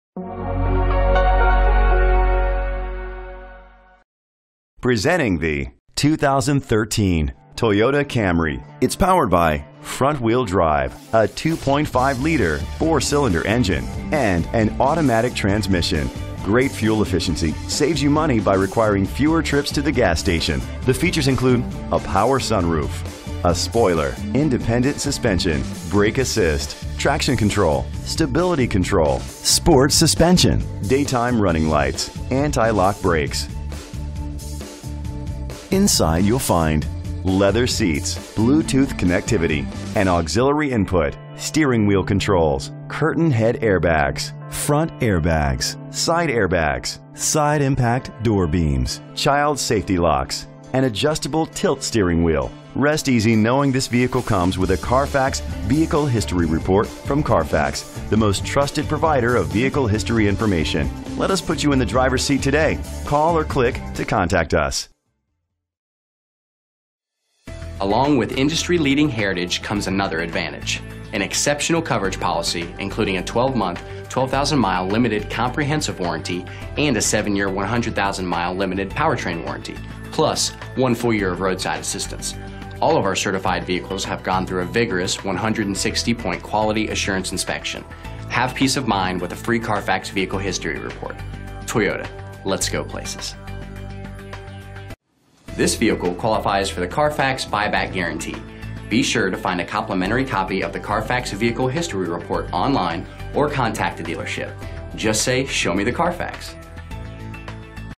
2013 Toyota Camry U379393A - Macon GA
Year: 2013
Make: Toyota
Model: Camry
Trim: SE
Engine: 2.5 liter 4 Cylindercylinder FWD
Transmission: Shiftable Automatic
Color: Black
Mileage: 77116
Address:
4580 Riverside Drive
Macon, GA 31210
VIN:4T1BF1FK4DU281946